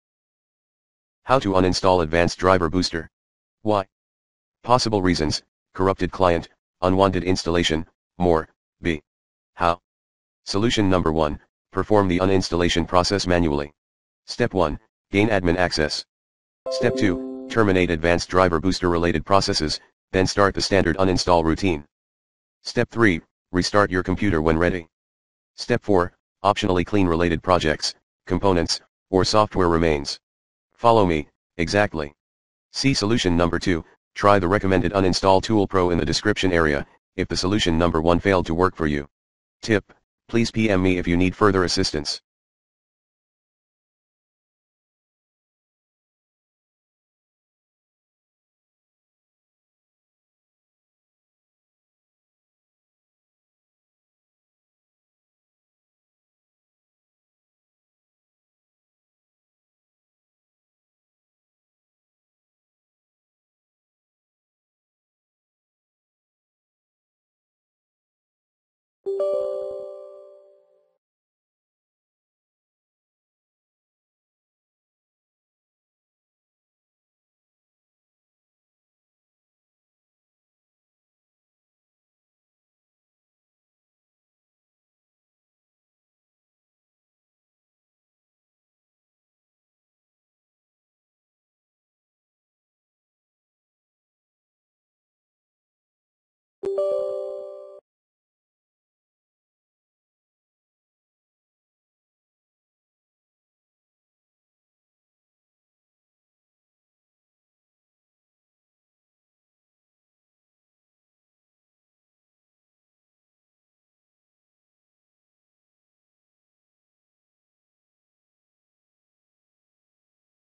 Remove Advanced Driver Booster in Windows 10 (Advanced Driver Booster Uninstall)
Here's how to uninstall/Remove Advanced Driver Booster. If you just can't do that, try the best uninstall tool@ Learn more?